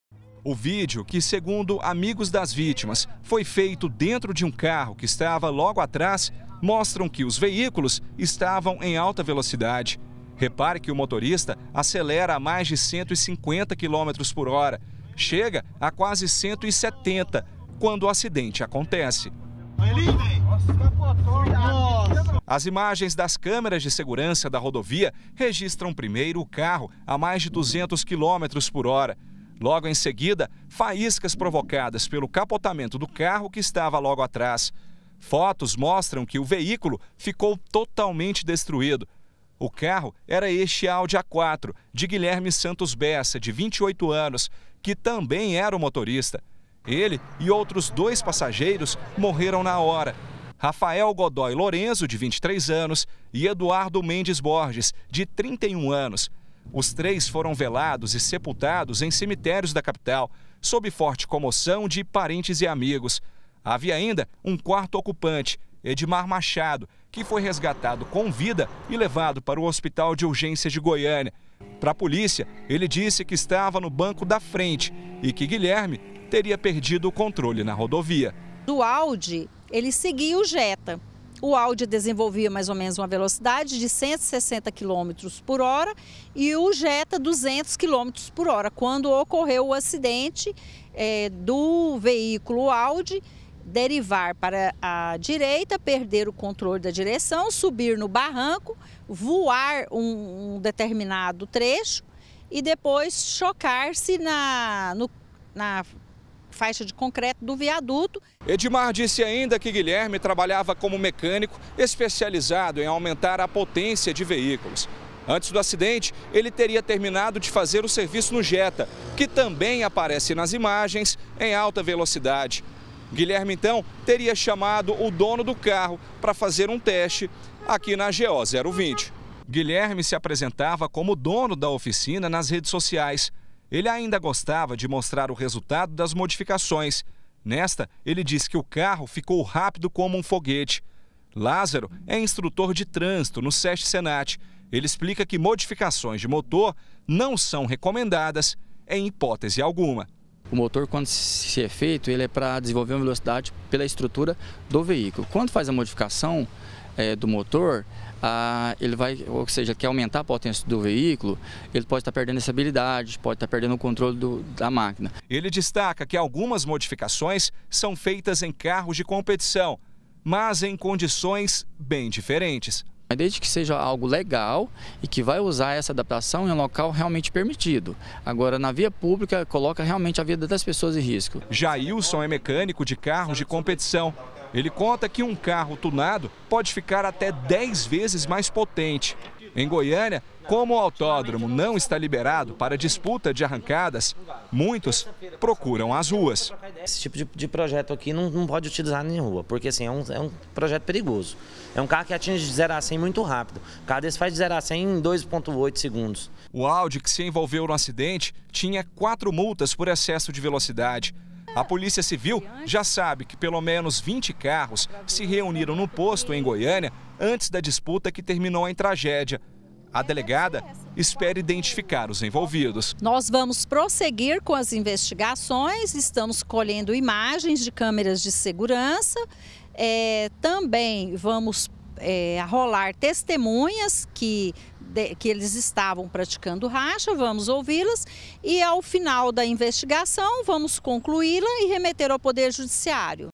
GA - Tragédia na GO-020: Polícia procura envolvidos - 24-05-2018
A polícia procura envolvidos em racha que terminou na morte de três jovens na GO 020, em Goiânia. Pelo menos vinte carros se reuniram num posto de gasolina antes da disputa que terminou em tragédia o carro teria sido modificado pra ficar mais potente. Técnica condenada por especialistas.